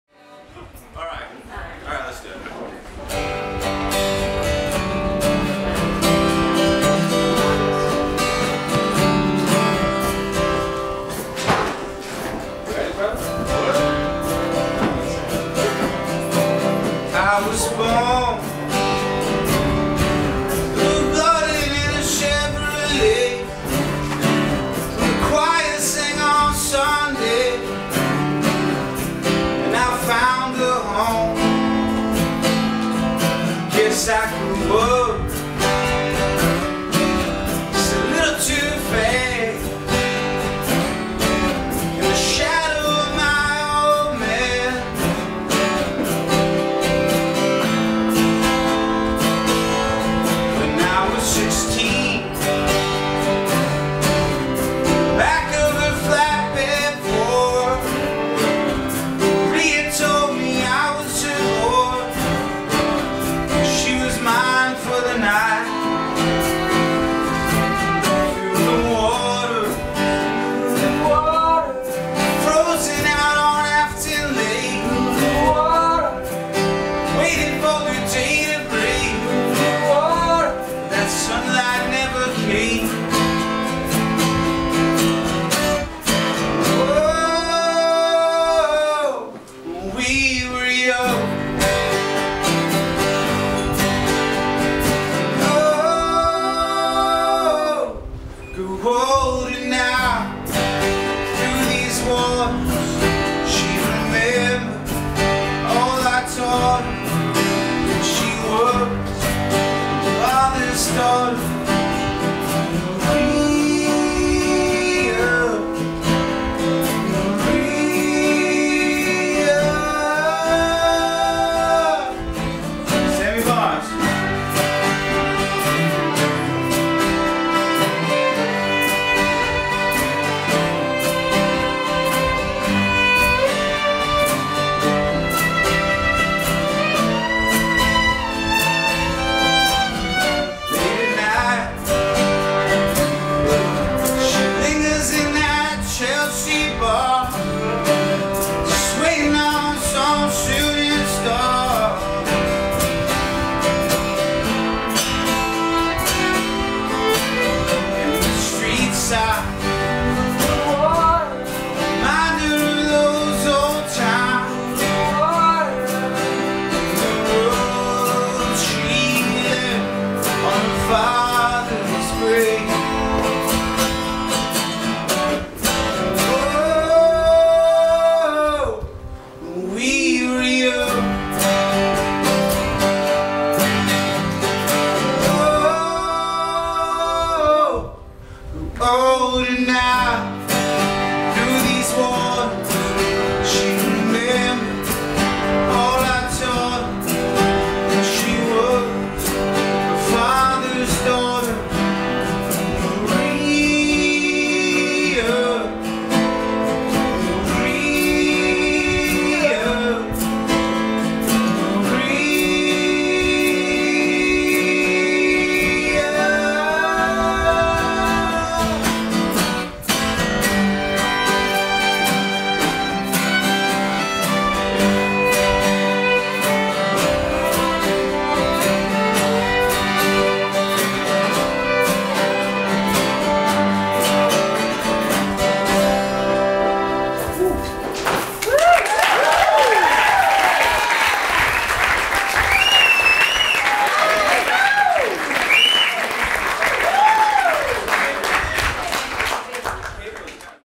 The Grand Nationals | Sofar NYC
The Grand Nationals performing at Sofar New York on March 23rd, 2012. We put on more than 10 shows every month in New York, see upcoming dates and sign-up for tickets

Artist: The Grand Nationals